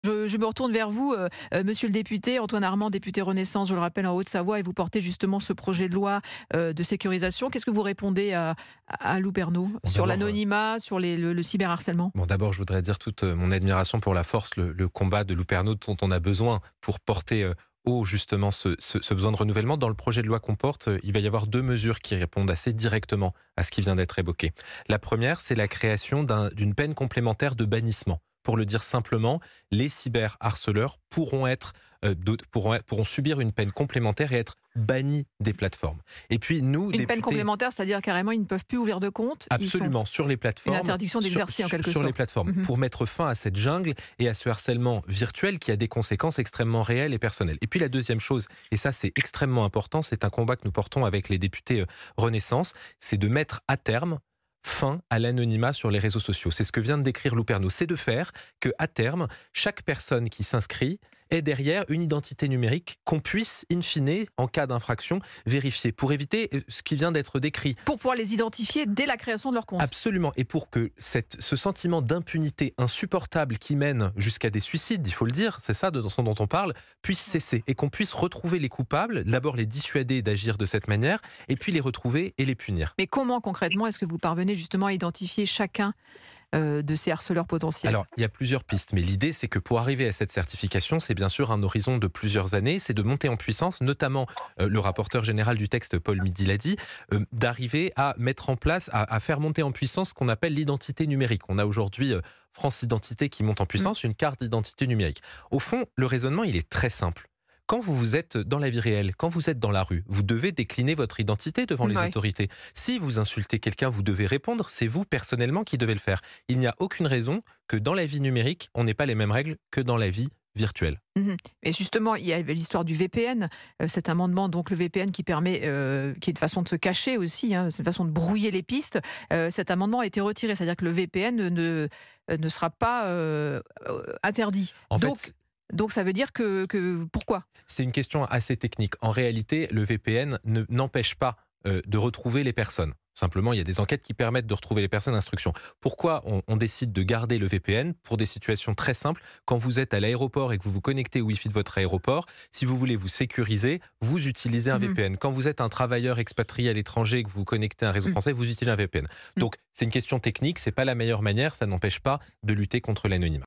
Peine complémentaire, anonymat, VPN... Les contours du projet de loi pour lutter contre le cyberh…
Peine complémentaire, anonymat, VPN... Les contours du projet de loi pour lutter contre le cyberharcèlement